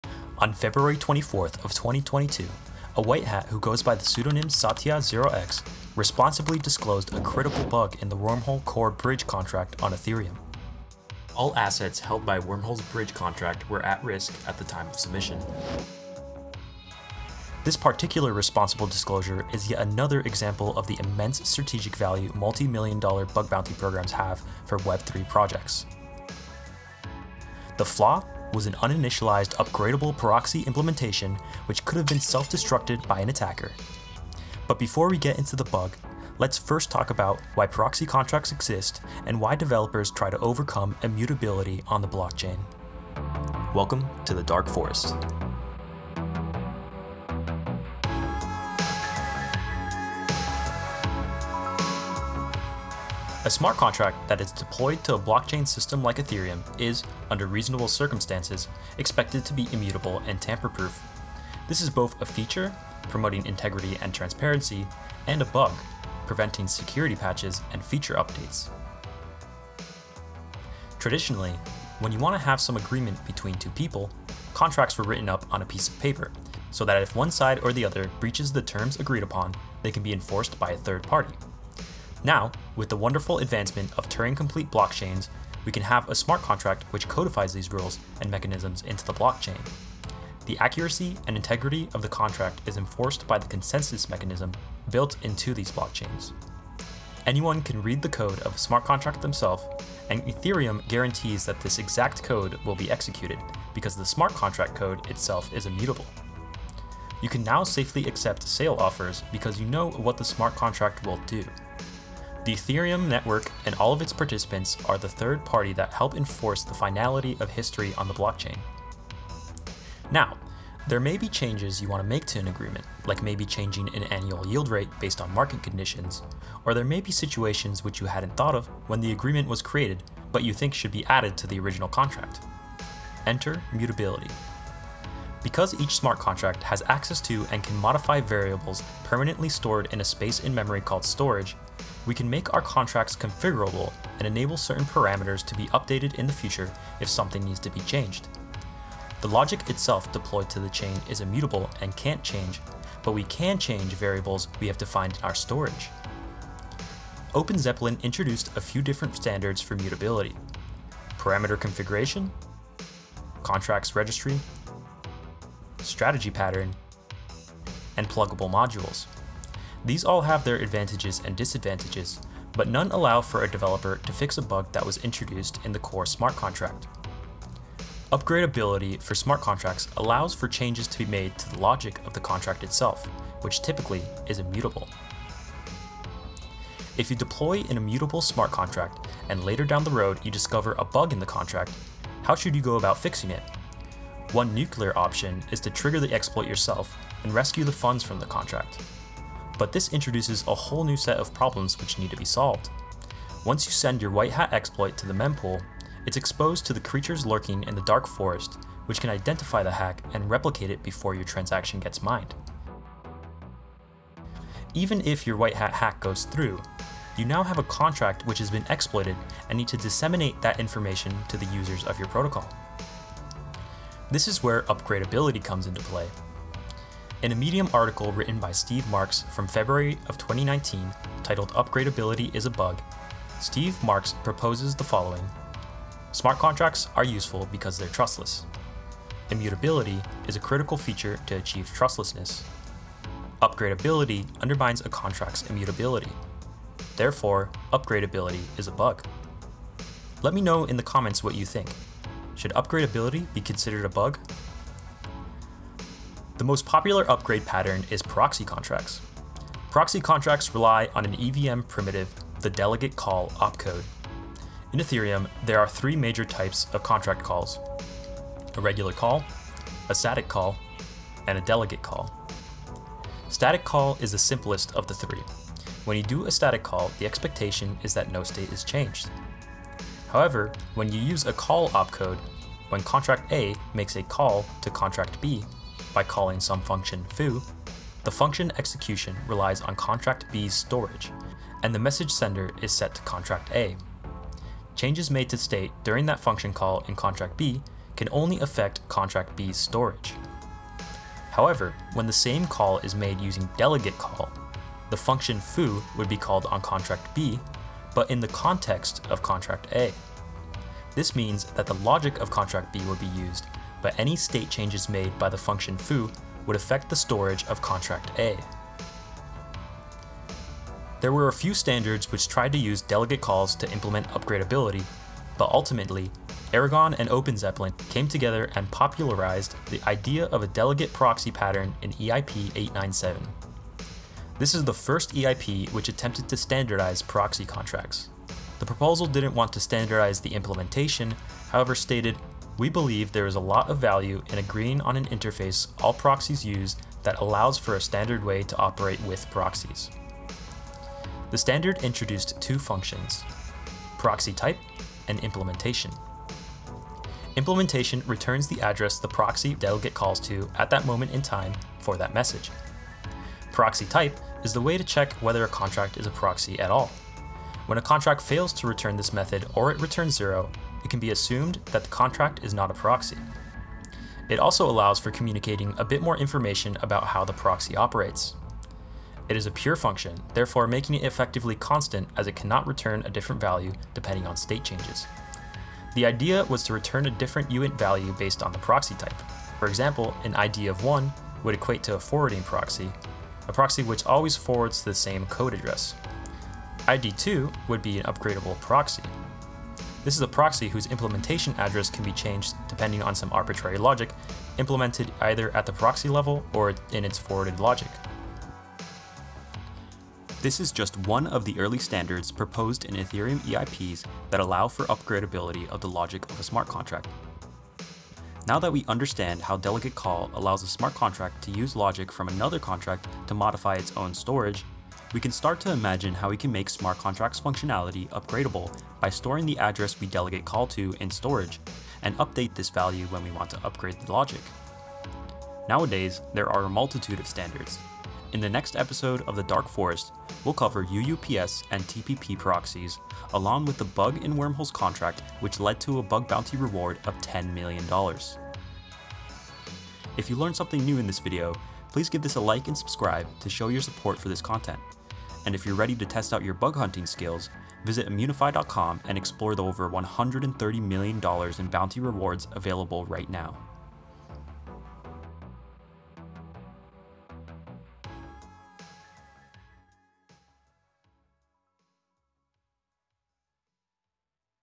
The Dark Forest EP1, Part 1: Wormhole Uninitialized Proxy
On February 24th, a whitehat who goes by the pseudonym satya0x, responsibly disclosed a critical bug in the Wormhole core bridge contract on Ethereum. This bug was an upgradeable proxy implementation self-destruct bug that could have led to a potential lockup of user funds. This responsible disclosure is yet another example of the immense strategic value that running a multi-million dollar bug bounty program can have for Web3 security programs.

Timeline

0:00 - Intro
0:28 - The bug
0:43 - Welcome to the Dark Forest
0:52 - Immutability
1:59 - Mutability
2:37 - OpenZeppelin mutability standards
3:05 - Upgradeability
4:15 - Proxies
5:20 - DelegateProxy
6:57 - Summary

Video producer: Alejandro Muños - Smart Contract Triager at Immunefi